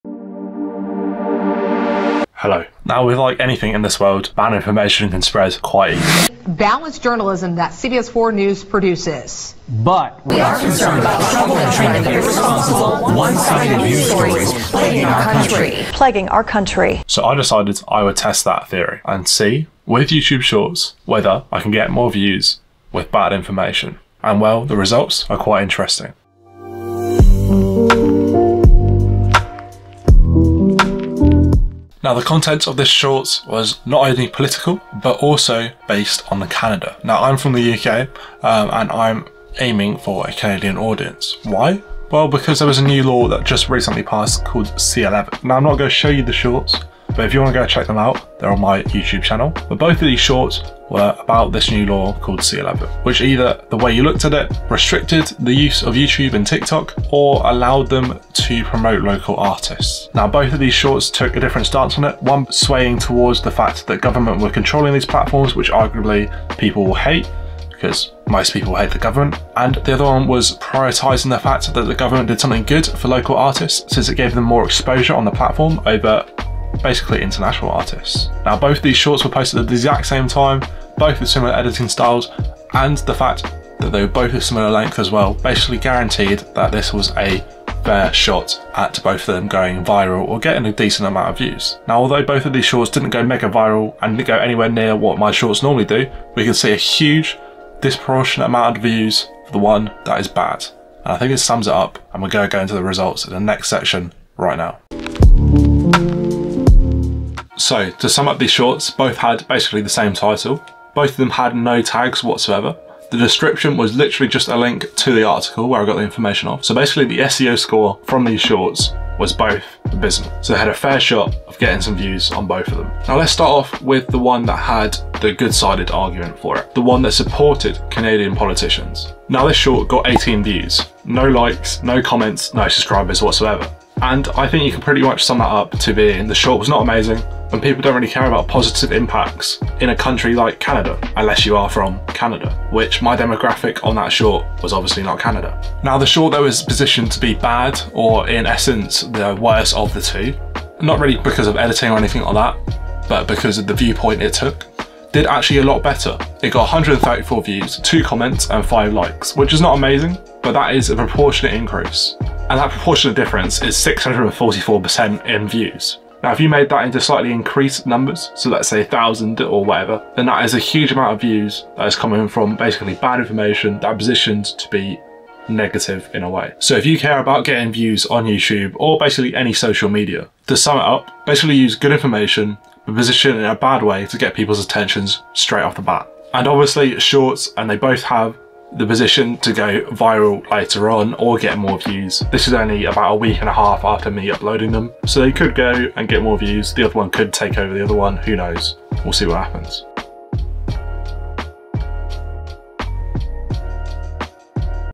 Testing YouTube AGAIN: Discover the Shocking Results!
Due to this video doing so well:
I decided to test YouTube again, to see whether I can fully understand how to get more views on YouTube Shorts. I think I managed it!